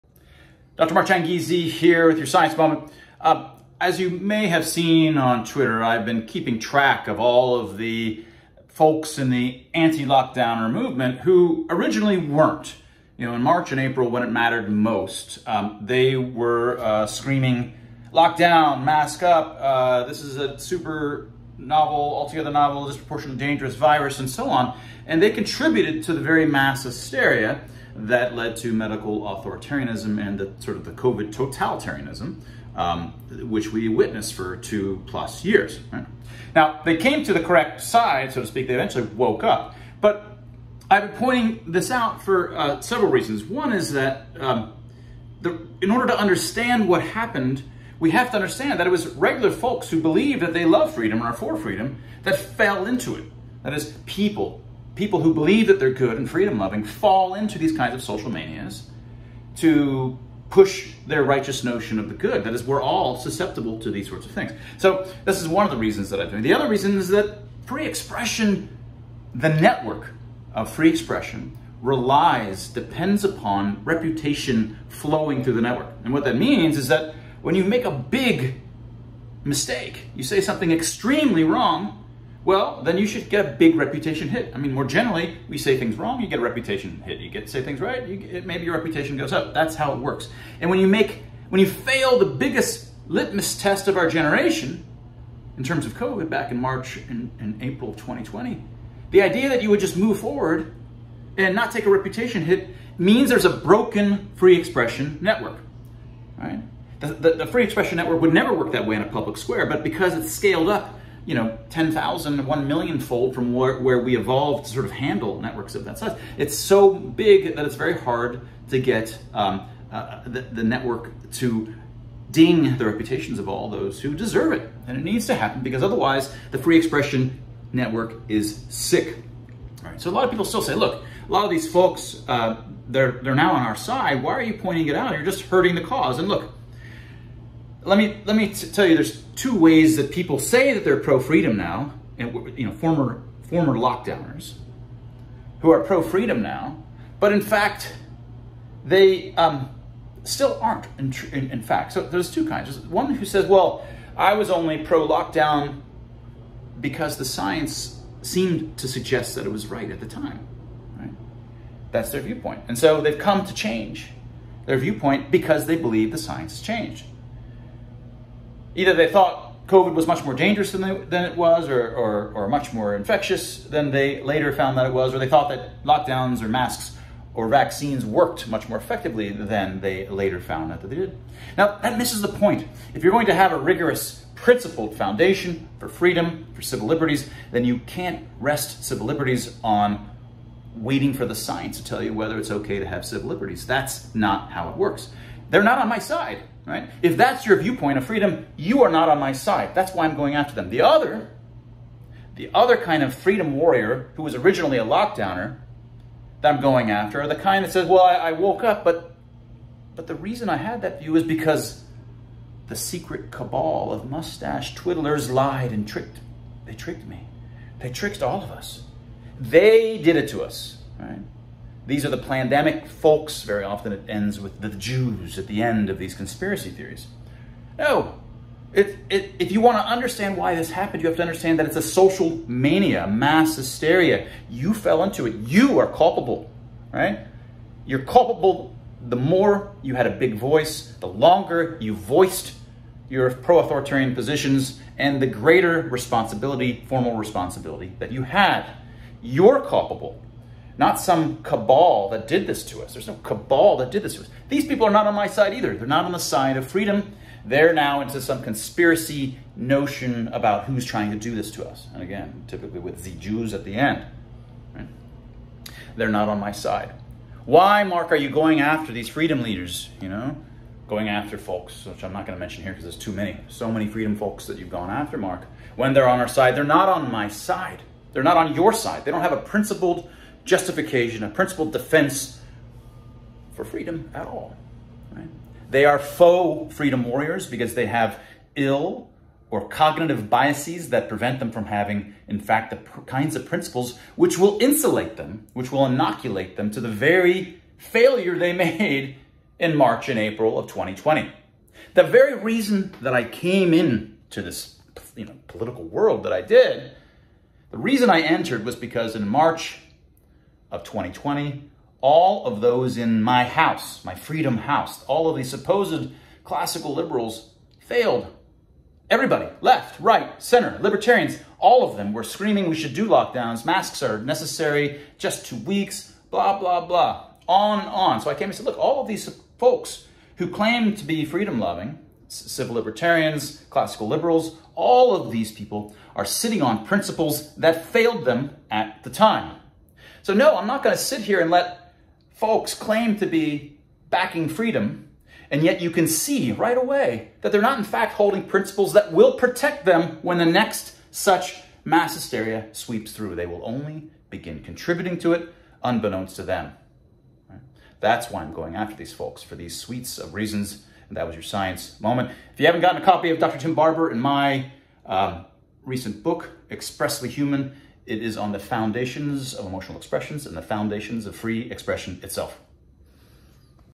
Why I go after so many fake “freedom leaders.” Moment 476
Long thread of anti-Lockdowners who started as Lockdowners.